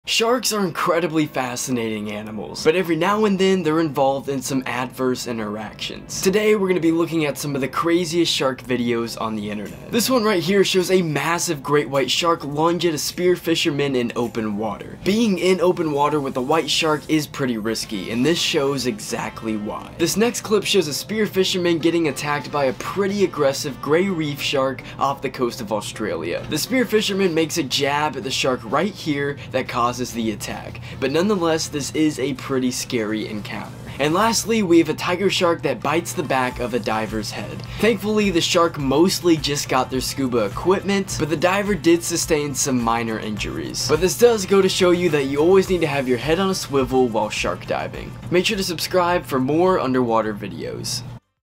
CRAZY Shark Attack Caught On Spear Fisherman
Three crazy shark encounters caught on film! Great White Shark, Reef Shark, and Tiger Shark!